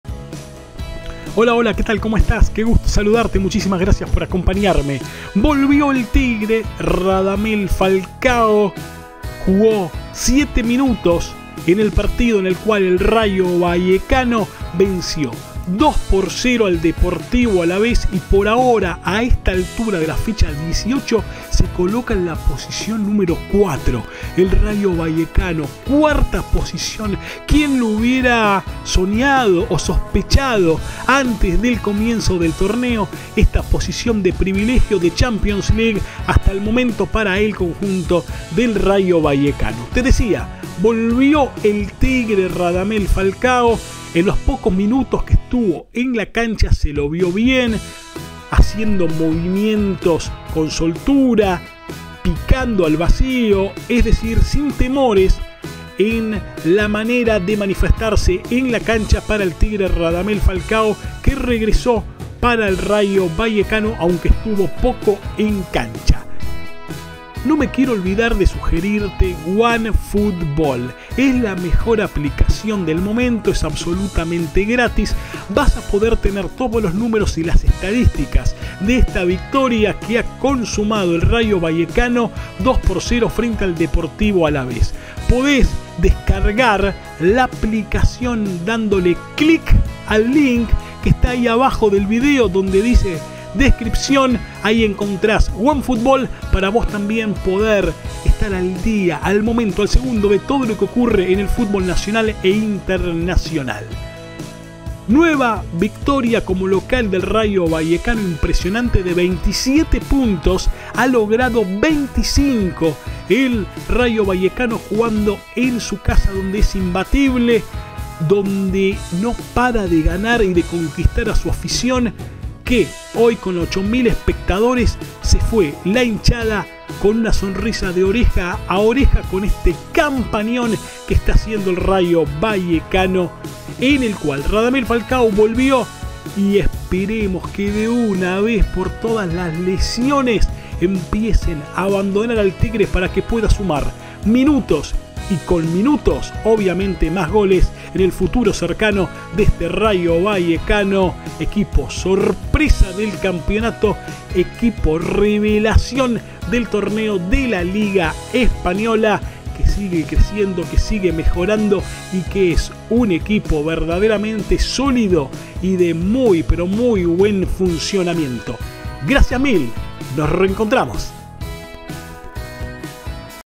REGRESA RADAMEL FALCAO⚽RAYO VALLECANO GANA Y ES IMBATIBLE COMO LOCAL
Regresa Radamel Falcao. Rayo Vallecano 2 Alaves 0